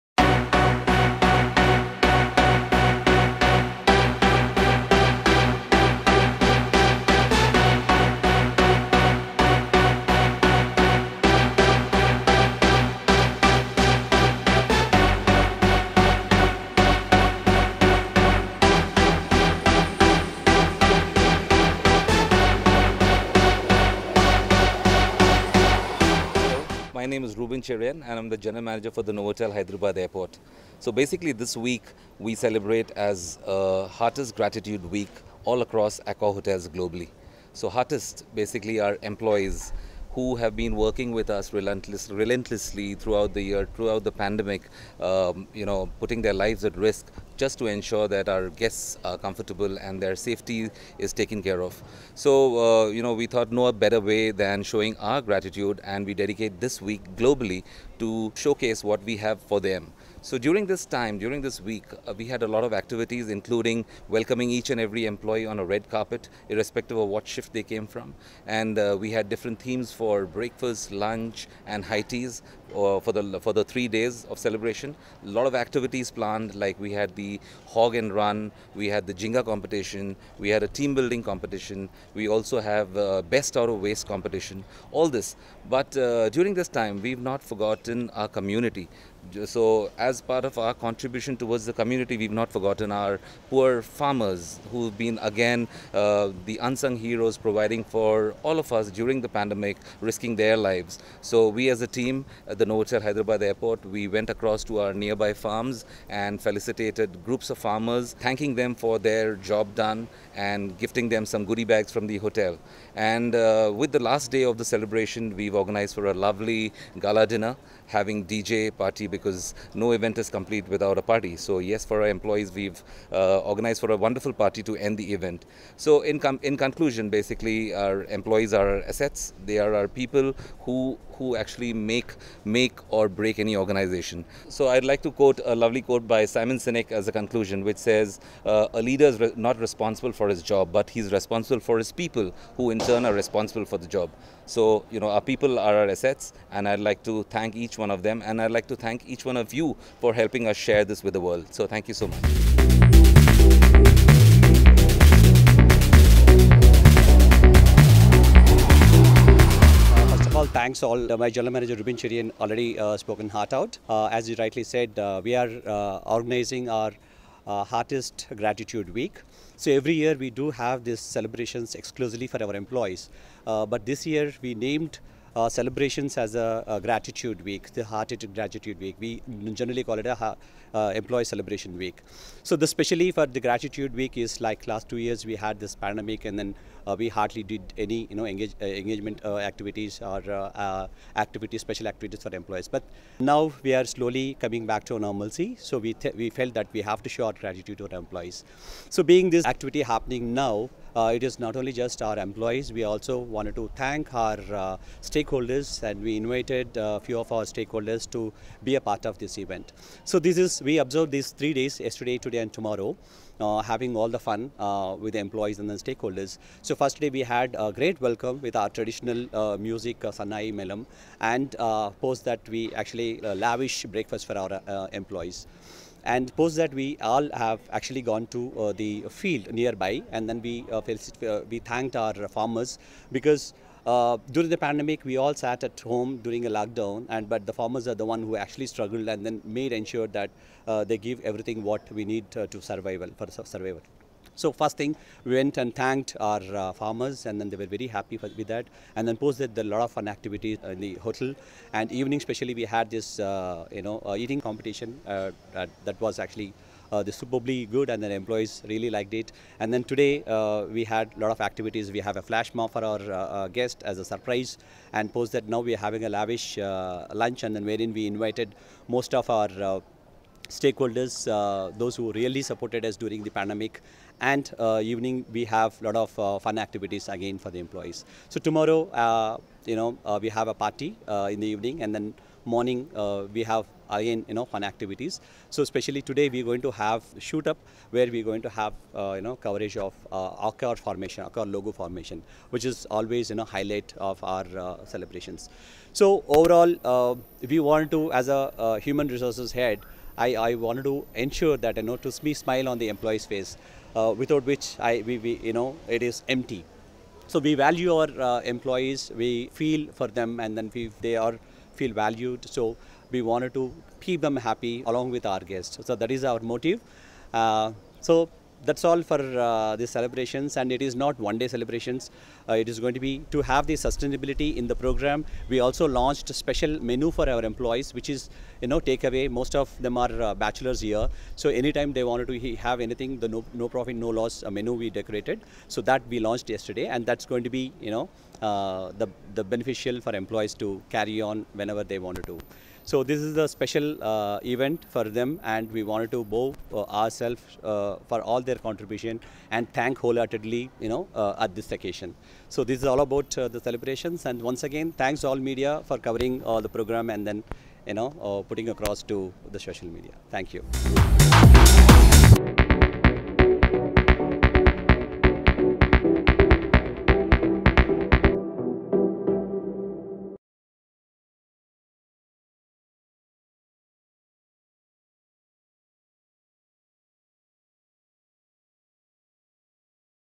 Novotel Hyderabad Airport Celebrates Heartiest Gratitude Week 2021 || Hybiz tv
Novotel Hyderabad Airport organized Heartiest Gratitude Week 2021 with an aim to acknowledge and recognize the employee’s relentless efforts in delivering highest standards of service contributing towards the success of the hotel. The name of this event this year is being changed from celebration to Heartiest Gratitude Week 2021 with a sole intention to recognize and appreciate Heartiest who have served selflessly and support and provide their service during thus pandemic. The celebration was done for three long days at Novotel Hyderabad Airport